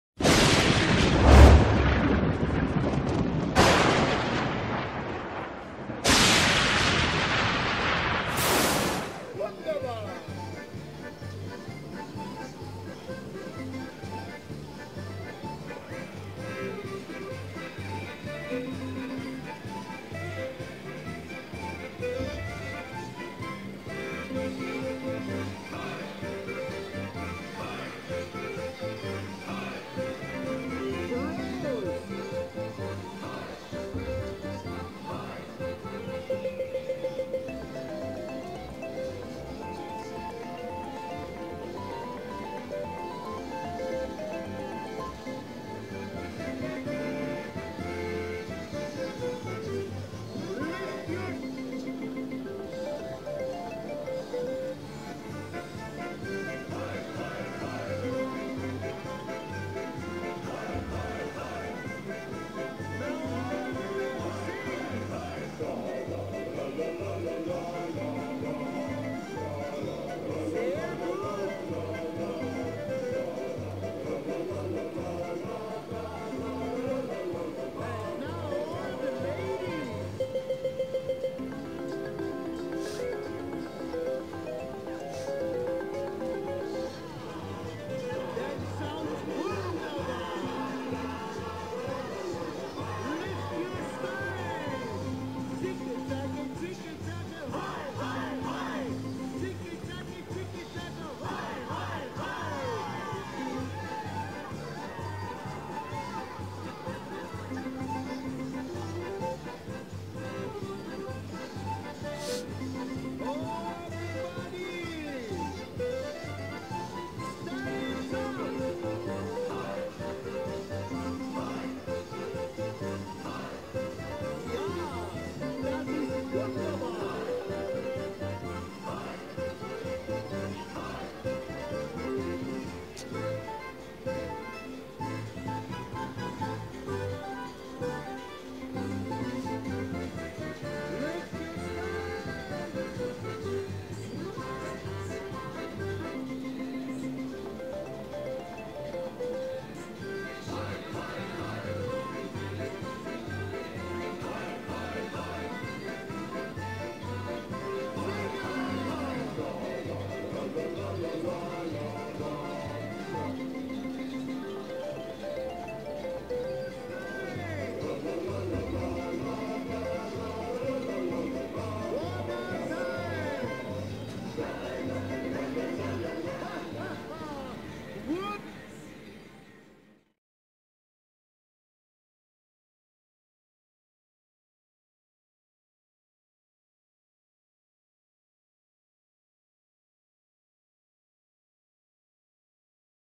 WMS - Bier Haus 200 - 30 Free Spin Bonus on $1.20 bet
Slot Name:  Bier Haus 200
Bet: $1.20 bet
Slot Maker: WMS
Filmed Location: Barona Casino & Resort
Description of video:   i put in a $10.00 in as i all way do and i hit the bonus before i busted.